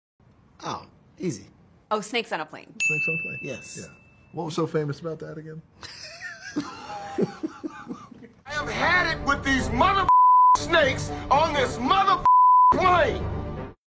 Dwayne Johnson, Kevin Hart, And Vanessa Bayer Play Our Emoji Game! 🤣
Dwane Johnson, Kevin Hart, And Vanessa Bayer Play Our Emoji Game!